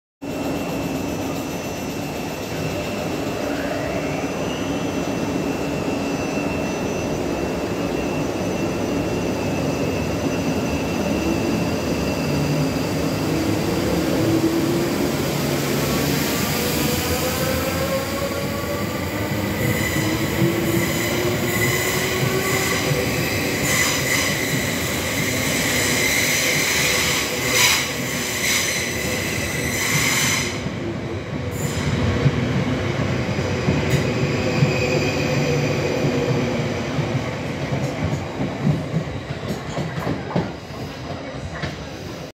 South Western Railway Class 458 departing Staines | 27/10/2023
South Western Railway Class 458 departing Staines

Operating a Westbound service to Reading